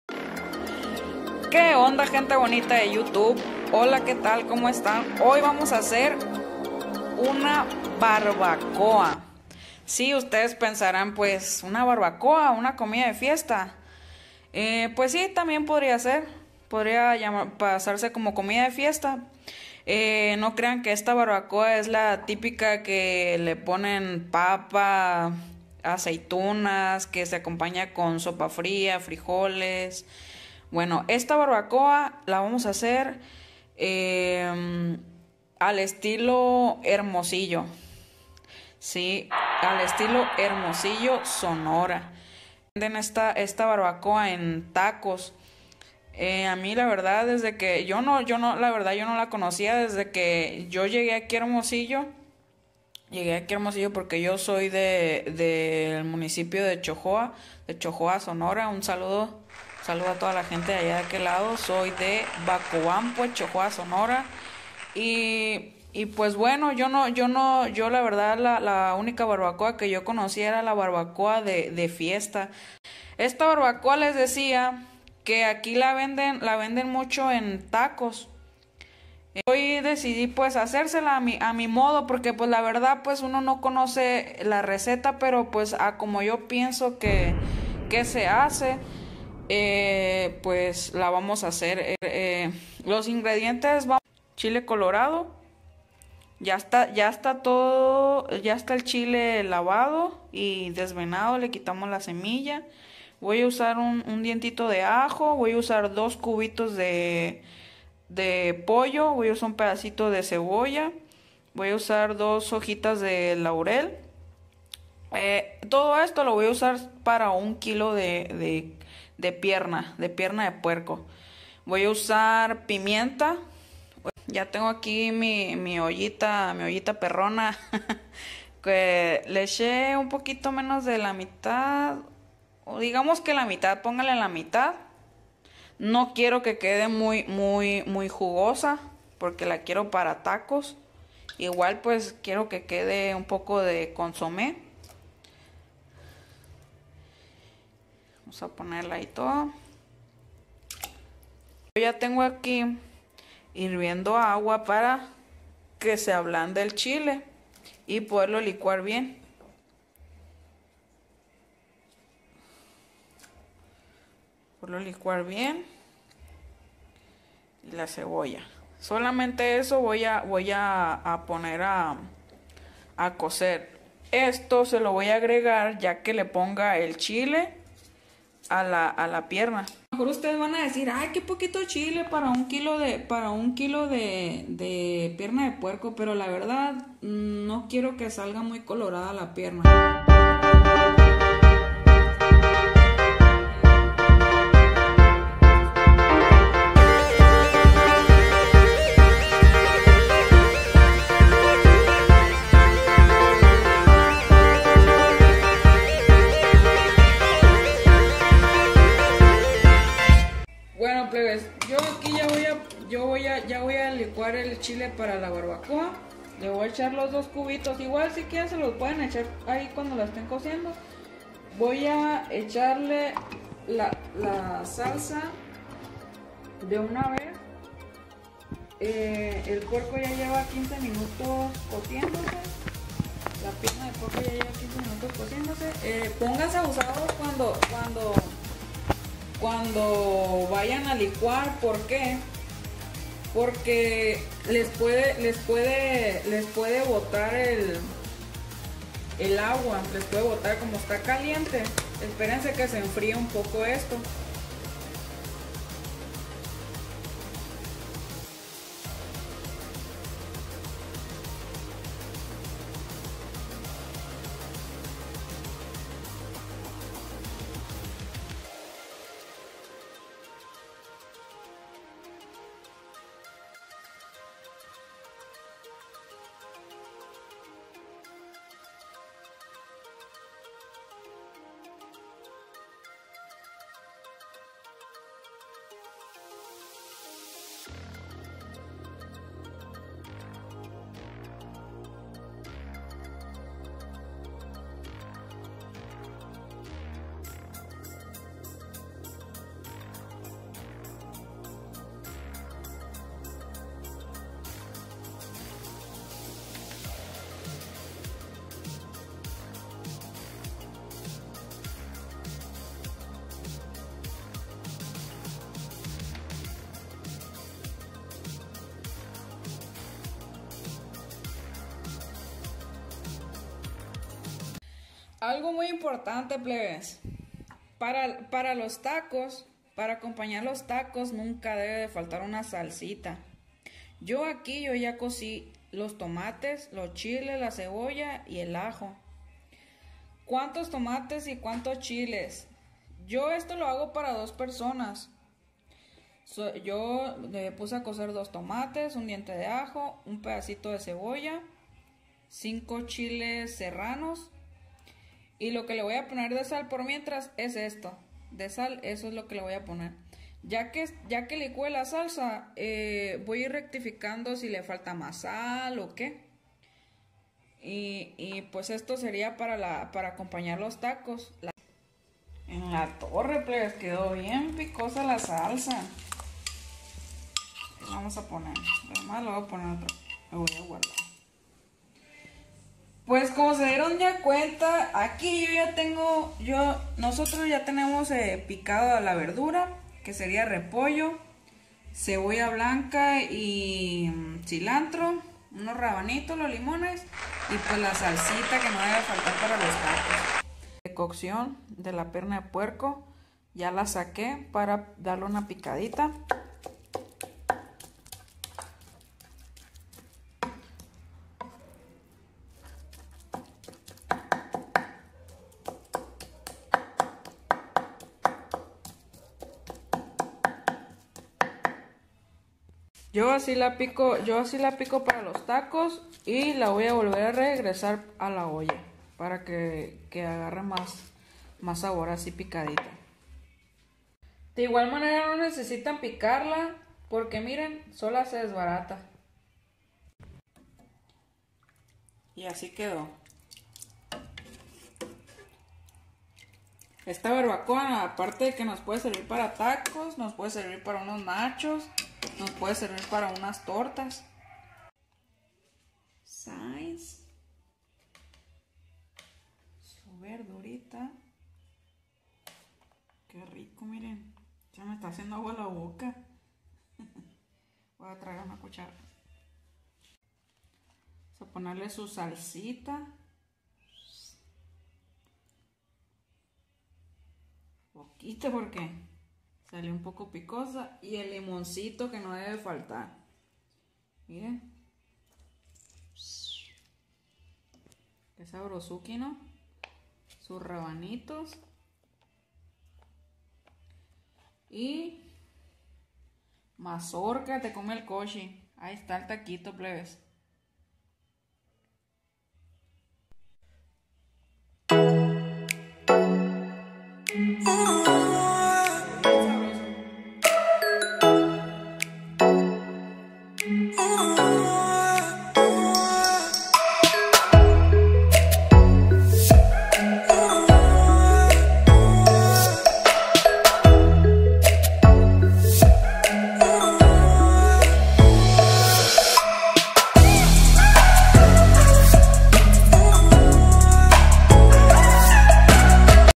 barbacoa a mi estilo ✌️ desde Hermosillo Sonora
la barbacoa en Hermosillo se prepara de res, está vez yo la prepare con pierna de puerco y a mi estilo!! yo no sé la respeta correcta pero igual sirve para lo mismo para hacerce unos tacos ó tortas ☺️